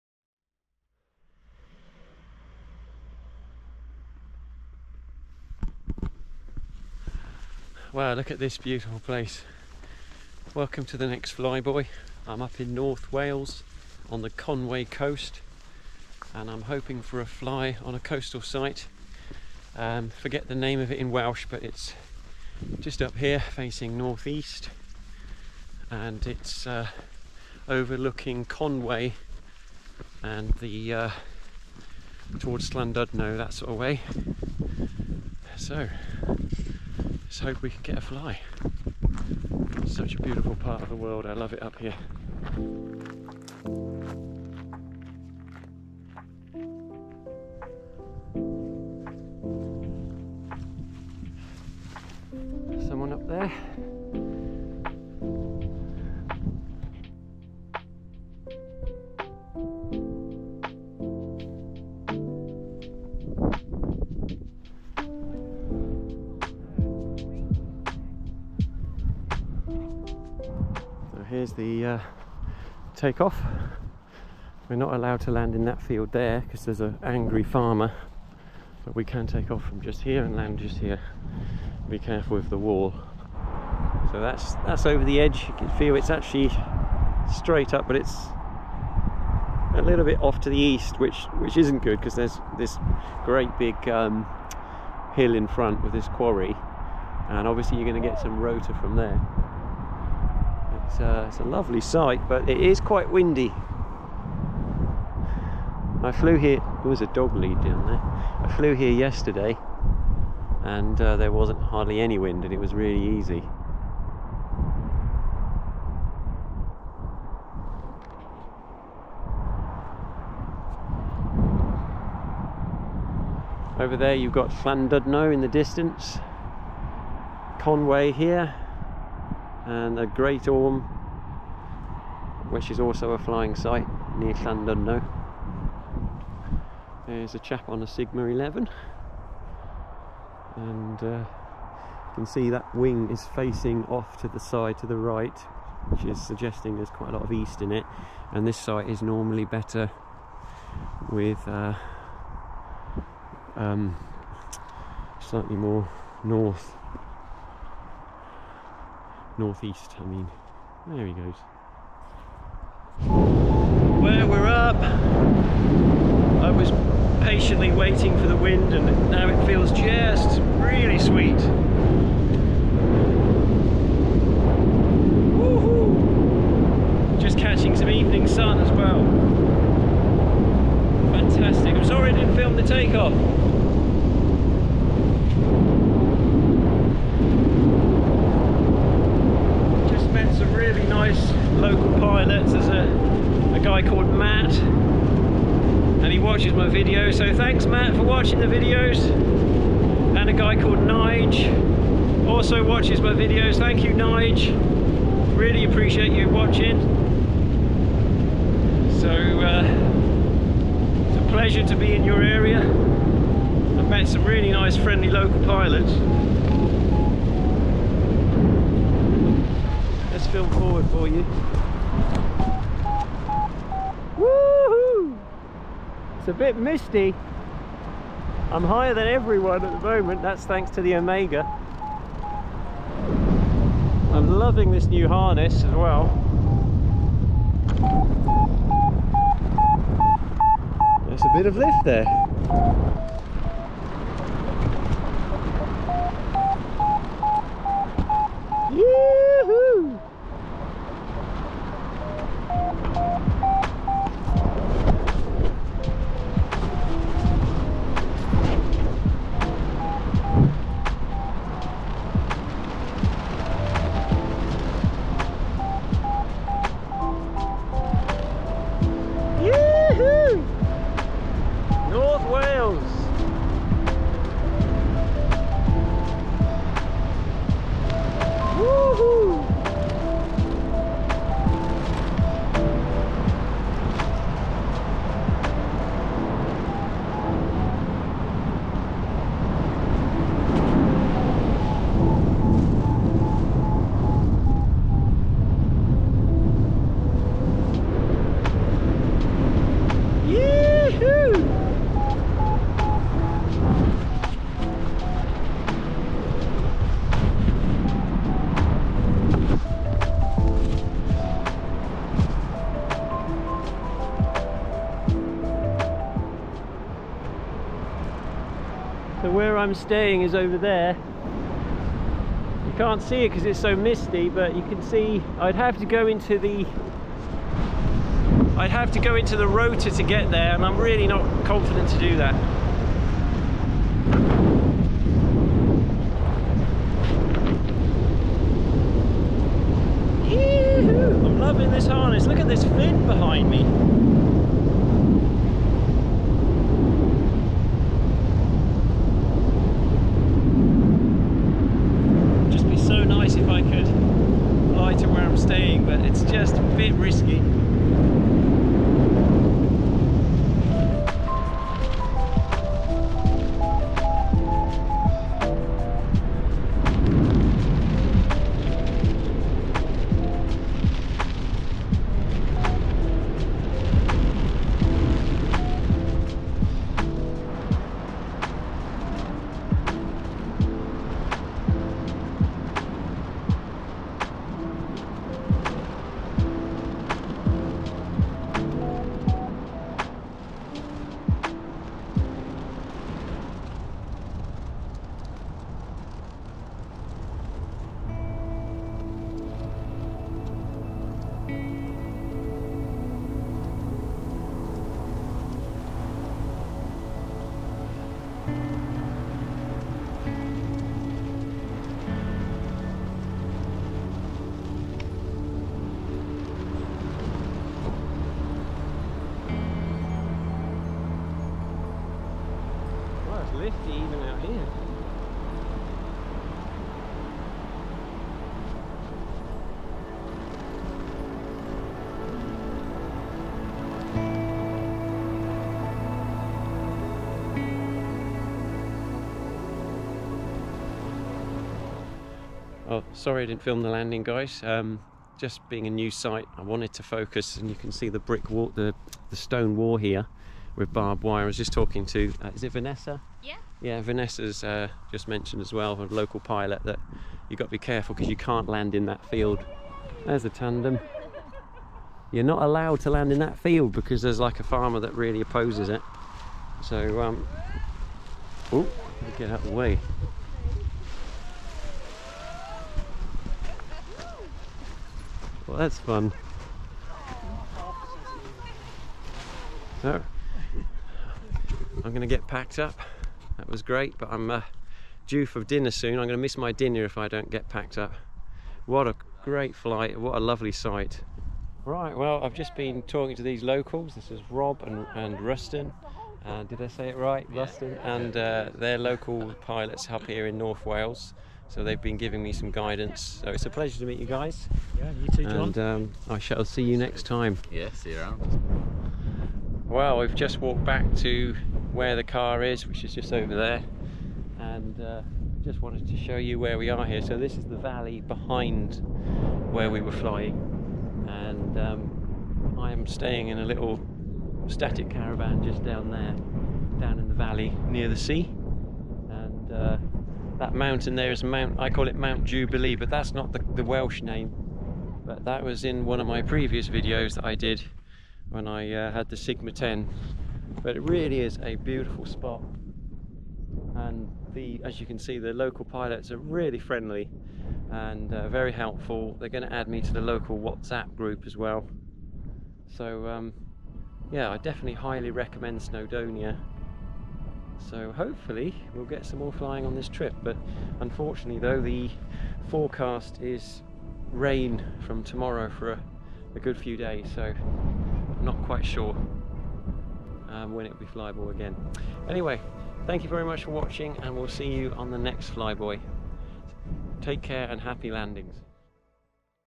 Fly Boy - Episode 63 - Paragliding in North Wales
Join me on this fantastic coastal flight in North Wales!

This is the first time I flew with my new harness, the BogdanFly 'Pizza Race' purchased from Ali Andrews (Crickhowell Paragliding).